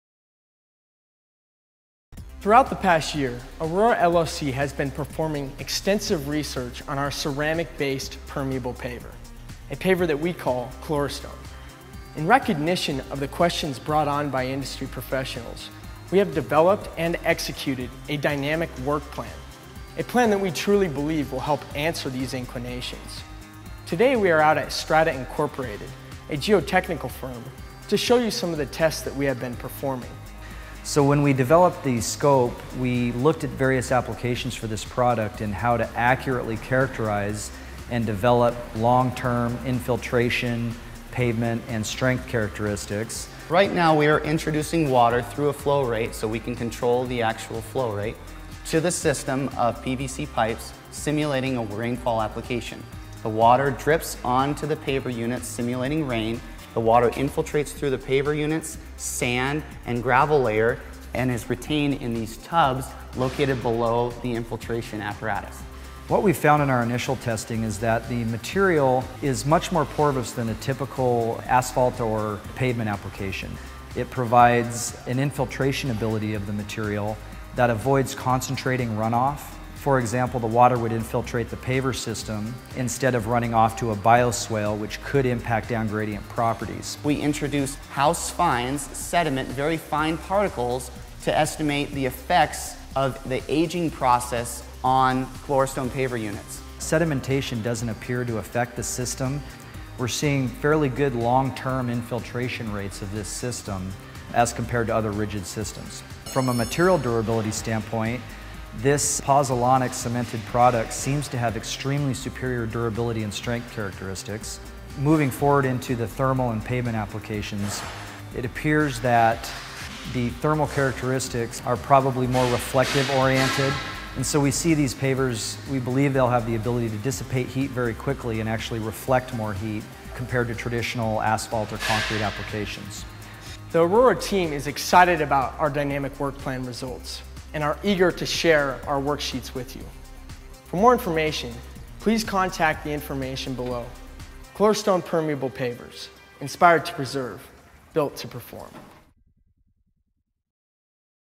Klorostone Water Infiltration Testing Web Video
I produced a video for Aroura LLC of water infiltration testing of Aroura LLC's permeable paver product, Klorostone.